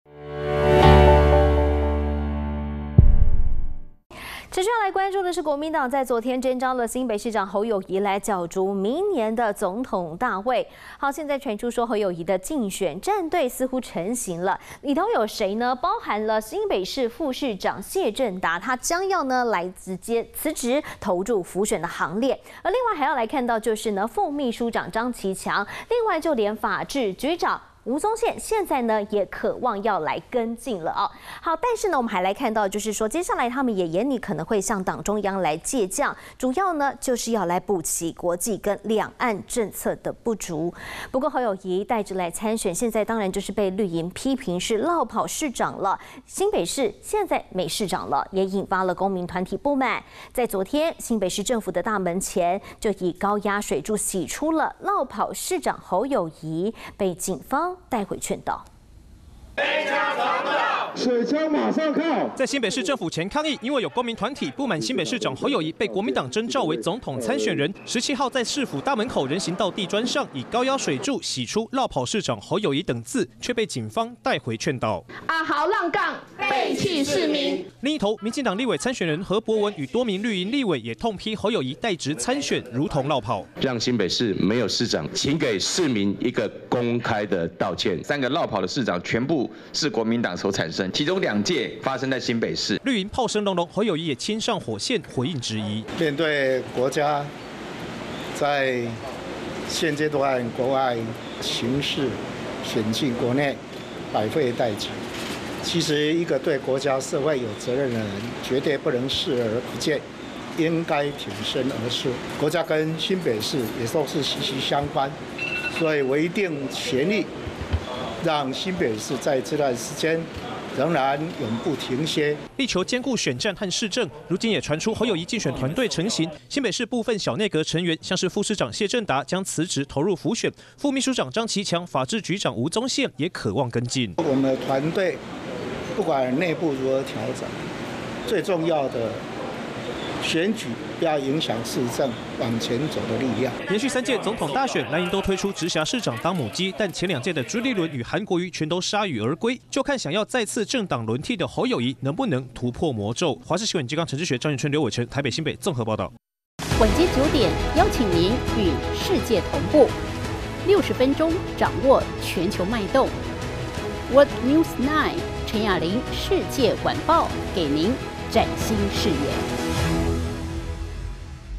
「侯戰隊」成形 新北副市長領軍率將辭職入競辦｜華視新聞 20230518
國民黨昨(17)日徵召新北市長侯友宜，角逐明年總統大位，傳出侯友宜競選戰隊成形，新北市副市長謝政達將辭職投入輔選，副祕書長張其強、法制局長吳宗憲也可望跟進。不過侯友宜帶職參選，被綠營批評落跑，讓新北市沒有市長，也引來公民團體不滿，昨(17)日在新北市政府大門前前，以高壓水柱洗出「落跑市長侯友宜」，卻被警方帶回勸導。